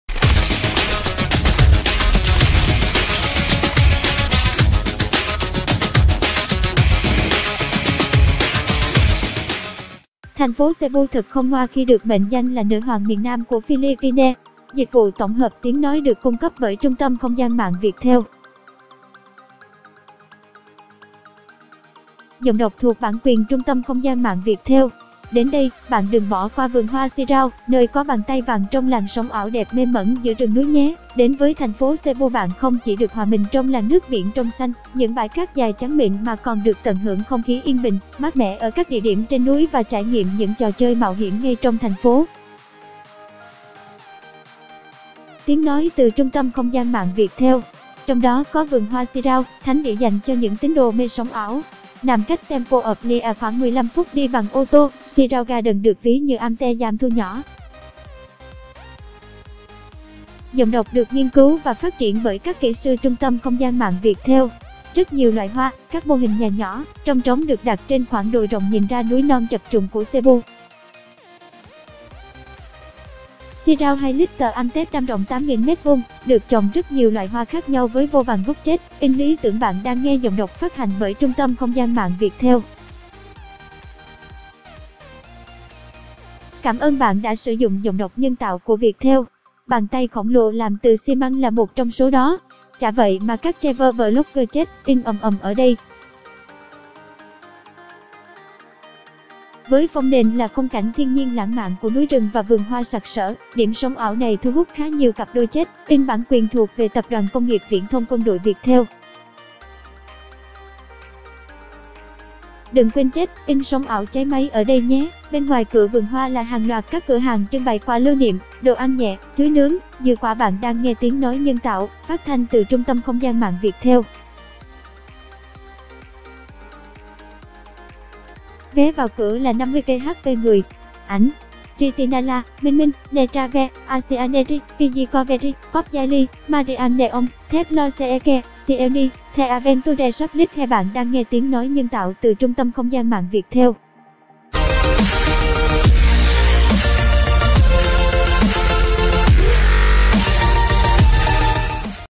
Tin Đc Ko - Thất điên bát đảo trước “Bàn tay vàng” trong làng “sống ảo” đẹp mê mẩn giữa rừng
Thành phố Cebu thật không ngoa khi được mệnh danh là “nữ hoàng miền Nam của Philipines”. Đến đây, bạn đừng bỏ qua vườn hoa Sirao, nơi có ‘Bàn tay vàng trong làng sống ảo’ đẹp mê mẩn giữa rừng núi nhé!   Đến với thành phố Cebu bạn không chỉ được hòa mình trong làn nước biển trong xanh, những bãi cát dài trắng mịn mà còn được tận hưởng không khí yên bình, mát mẻ ở các địa điểm trên núi và trải nghiệm những trò chơi mạo hiểm ngay trong thành phố. Trong đó có vườn hoa Sirao, thánh địa dành cho những tín đồ mê sống ảo.      Nằm cách Temple Of Leah khoảng 15 phút đi bằng ô tô, Sirao Garden  được ví như Amsterdamthu nhỏ. Rất nhiều loại hoa, các mô hình nhà nhỏ, chong chóng được đặt trên khoảng đồi rộng nhìn ra núi non trập trùng của Cebu.       Sirao hay Little Amsterdam rộng 8.000 m2, được trồng rất nhiều loại hoa khác nhau với vô vàn góc check-in lý tưởng. Bàn tay khổng lồ làm từ xi măng là một trong số đó. Chả vậy mà các travel blogger check-in ầm ầm ở đây.        Với phông nền là khung cảnh thiên nhiên lãng mạn của núi rừng và vườn hoa sặc sỡ, điểm sống ảo này thu hút khá nhiều cặp đôi check-in. Đừng quên check-in “sống ảo” cháy máy ở đây nhé!      Bên ngoài cửa vườn hoa là hàng loạt các cửa hàng trưng bày quà lưu niệm, đồ ăn nhẹ, chuối nướng, dừa quả. Vé vào cửa là 50 PHP/người.    Ảnh: Christinallar, Rei_minmin, Nextravelph, Asiascenery, Pinoydiscovery, Popdaily_hk, Marianne_ong, Theflowseekers, Sieuni_ , Theadventurersoflife